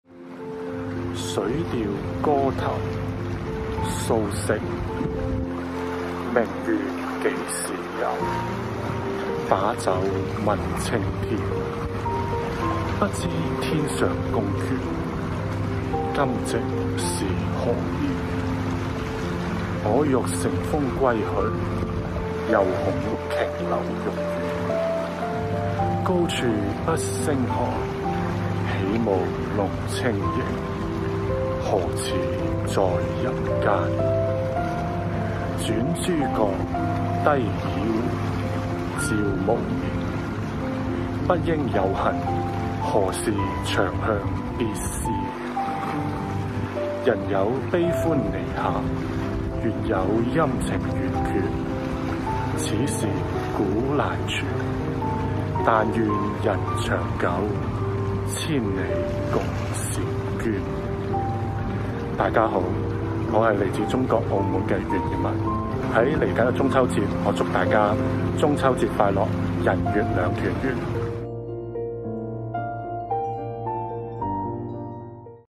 CGTN launches global 'Read a Poem' campaign- UN lo Man reads 'Prelude to Water Melody' in Cantonese
UN lo Man reads the famous "Prelude to Water Melody" by Su Shi in Cantonese as part of CGTN's "Read a Poem" campaign. Launched the special event for the Mid-Autumn Festival and International Day of Peace in September, we invite you to read a poem featuring the themes of love and peace in your native language. Your video could be re-posted by CGTN's official account. Don't forget to say our slogan in the video, "I read this poem for you, though thousands of miles apart, poetry brings us the same beat of the heart."